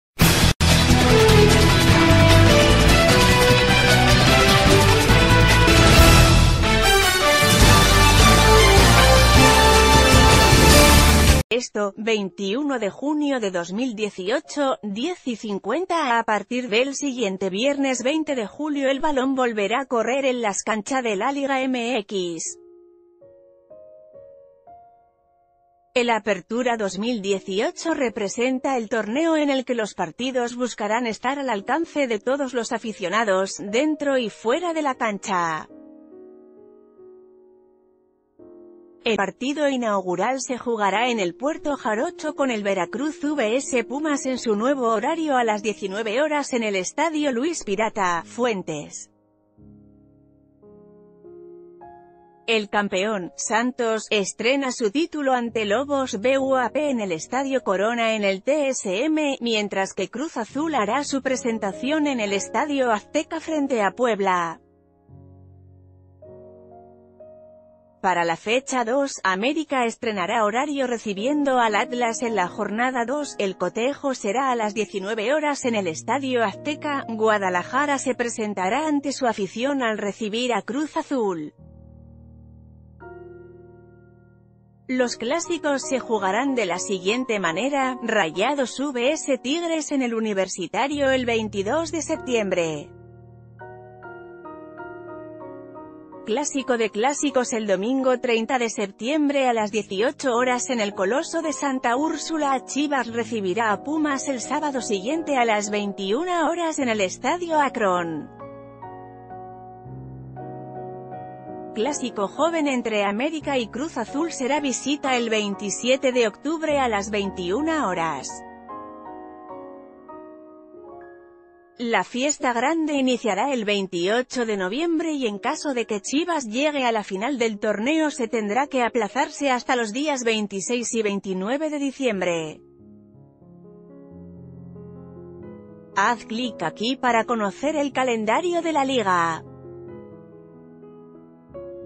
Listo calendario del Apertura 2018 de la LigaMx
ESTO | 21 Junio 2018 | 10:50 A partir del siguiente viernes 20 de julio el balón volverá a correr en las cancha de la LIGA MX. El Apertura 2018 representa el Torneo en el que los partidos buscarán estar al alcance de todos los aficionados, dentro y fuera de la cancha. El partido inaugural se jugará ...